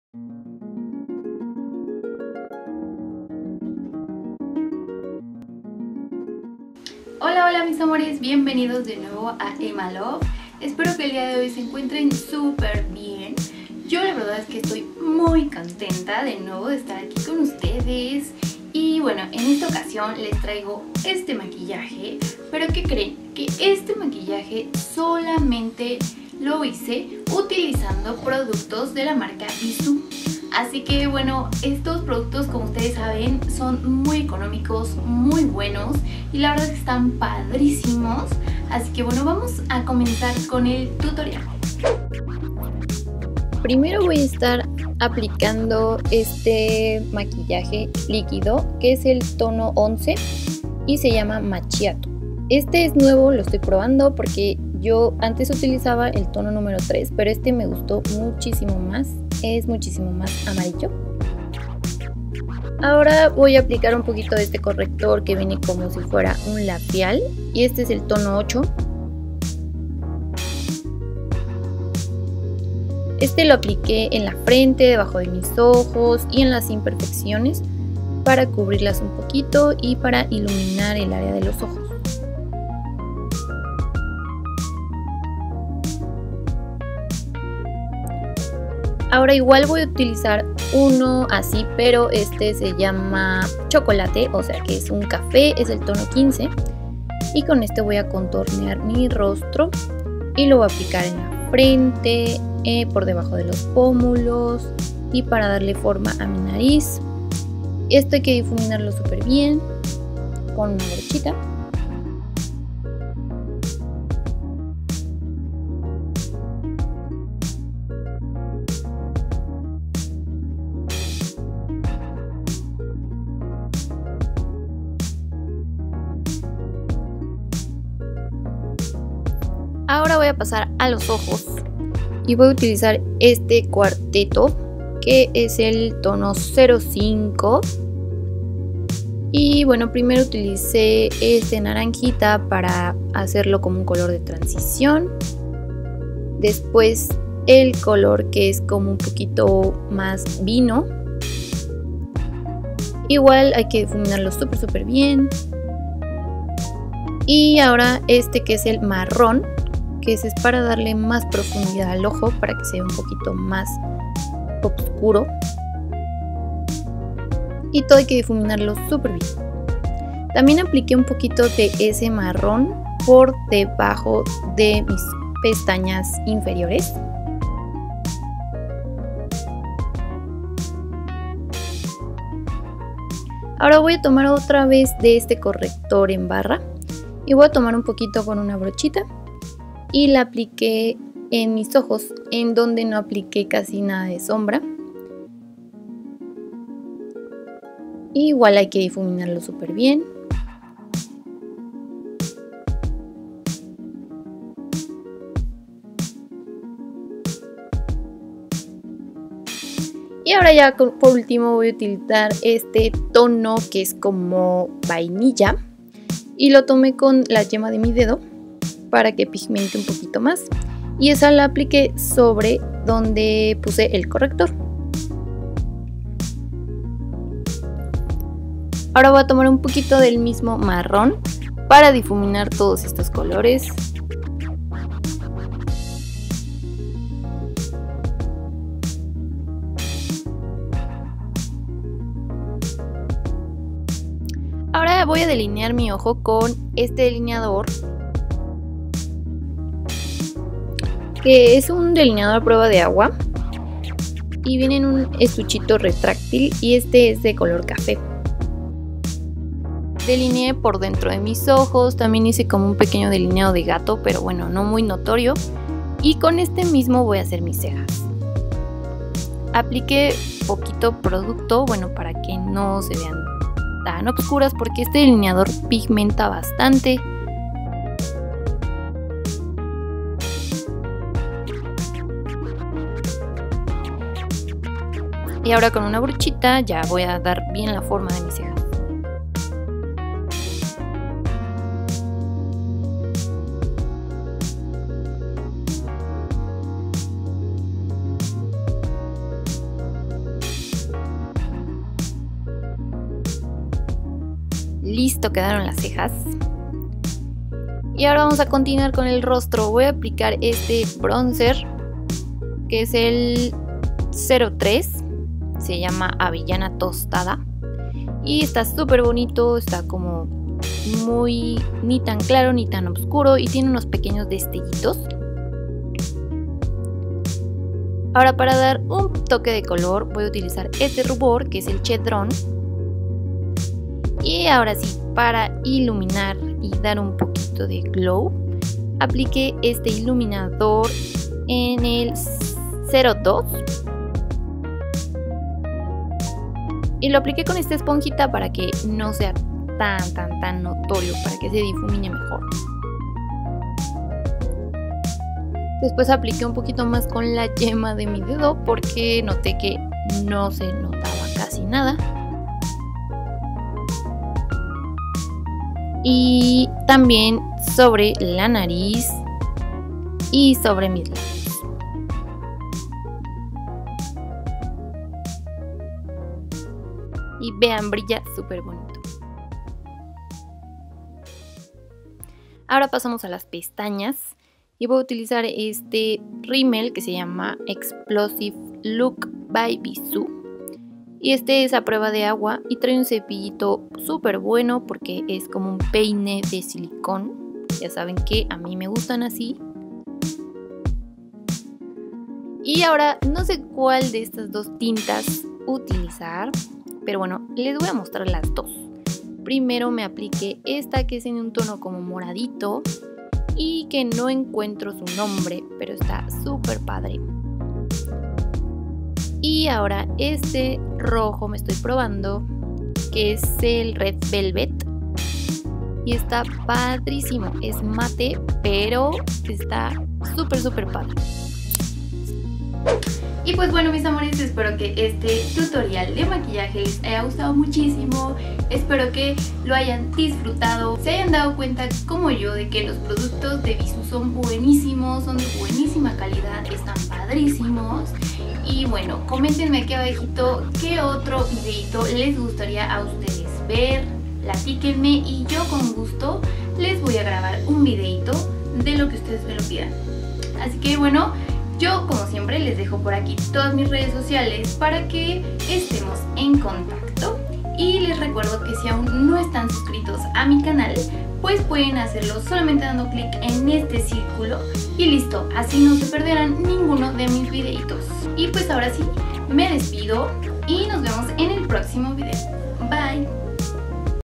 PRODUCTOS BISSU : TUTORIAL ⎮ EMMA LOVE
MAQUILLAJE COMPLETO PRODUCTOS BISSU !!!

CAJITA DE MAS INFO 💟

HOLA MIS AMOR@S BIENVENIDOS 🙋🏻💓💓💓
EN EZTE VIDEO LES MUESTRO UN MAQUILLAJE UTILIZANDO SOLO PRODUCTOS DE LA MARCA BISSÚ MARCA SUPER BUENA CON PRODUCTOS SUPER ECONOMICOS , UTILIZO TONOS MUY CALIDOS PARA QUE SEA UN LOOK QUE SE PUEDE USAR DE DIA O NOCHE.

Productos utilizados:

-Base Long Wear Foundation TONO:11
-Corrector Tono: Yellow
-Corrector Tono: Chocolate
-Cuarteto Tono: 05
-Delineador Waterproof Tono: Cafe
-Bronzer Tono:03 Avellana Tostada
-Rubor Tono: Chedron
-Iluminador
-Rimel o Mascara Tono: Negro
-Tinta Matte Tono: Intuicion
-Tinta Matte 2 Tono: Red Velvet

COMPARTELO Y DAME LIKE 👍
LOS QUIERO MUCHISIMO AMORES💓💓💓💓💓

SIGUEME PARA TODOS LADOS.